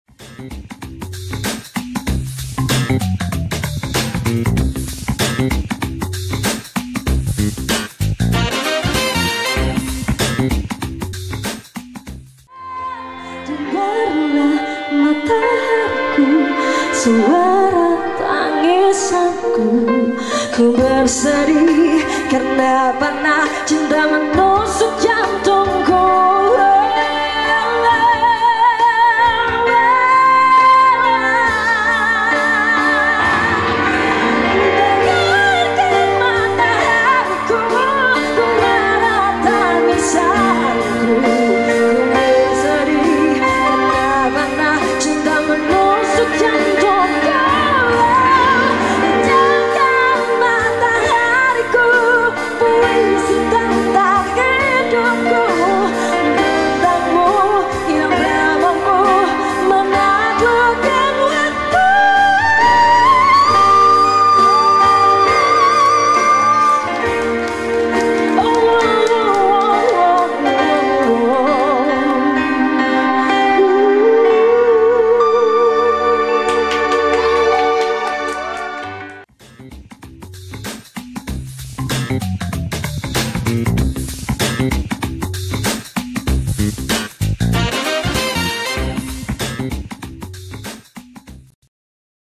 triemusic spontanitas VINA pengunjung @Gracia Hot Spring
kami rutin mengisi acara Live Music di gracia hot spring water setiap sabtu malam dan malam liburan..datang yah ke Gracia
kontak kami di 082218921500 & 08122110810